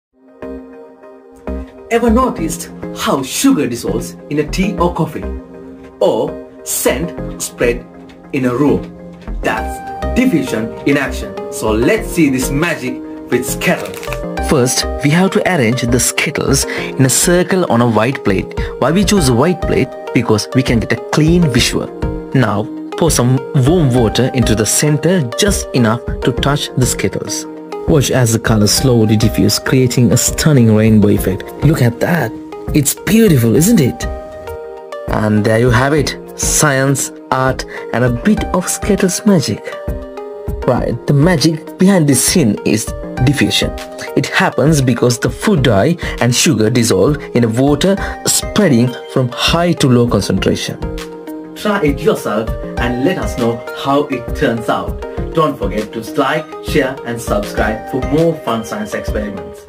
Diffusion🔥 |Skittles🌈🌈 |candy science
Hey everyone! Today, we're diving into some sweet science—literally! 🍬 We're about to show you an epic candy experiment that'll blow your taste buds and your mind! 🧪✨ Try it out, and don’t leave us hanging—drop your colorful results in the comments below. Let’s make this a rainbow of fun!🌈